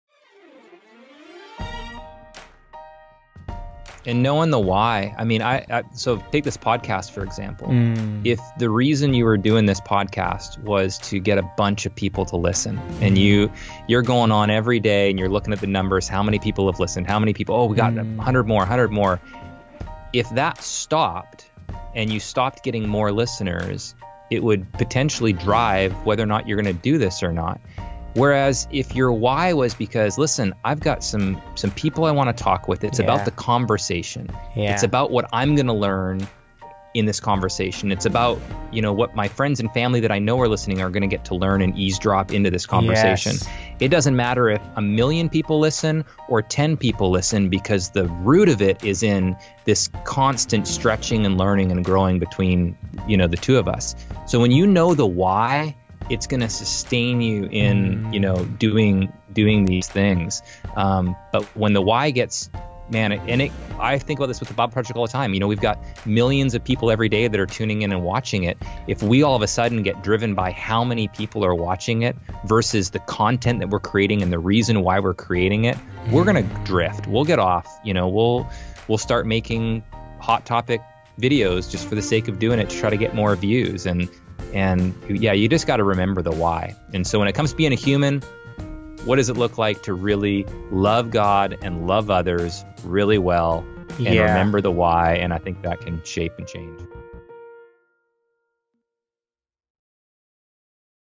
"...And Knowing the Why" — Ep#13 Highlights with Michael McDonald
Michael shares about the importrance of knowing your why when it comes to doing justice work.

"And knowing the why I mean, so take this podcast, for example, if the reason you were doing this podcast was to get a bunch of people to listen, and you, you're going on every day, and you're looking at the numbers, how many people have listened, how many people Oh, we got 100, more 100 more. If that stopped, and you stopped getting more listeners, it would potentially drive whether or not you're going to do this or not. Whereas if your why was because, Listen, I've got some, some people I want to talk with, it's about the conversation. It's about what I'm going to learn in this conversation. It's about, you know, what my friends and family that I know are listening are going to get to learn and eavesdrop into this conversation. It doesn't matter if a million people listen, or 10 people listen, because the root of it is in this constant stretching and learning and growing between you know, the two of us, so when you know the why it's gonna sustain you in you know, doing doing these things. But when the why gets man in it, I think what this what the Bible Project all the time, you know, we've got millions of people every day that are tuning in and watching it, if we all of a sudden get driven by how many people are watching it, versus the content that we're creating, and the reason why we're creating it. We're gonna drift we'll get off you know, we'll, we'll start making "Hot Topic" videos just for the sake of doing it try to get more views and, and yeah, you just got to remember the why. And so when it comes to being a human, what does it look like to really love God and love others really well. And remember the why and I think that can shape and change."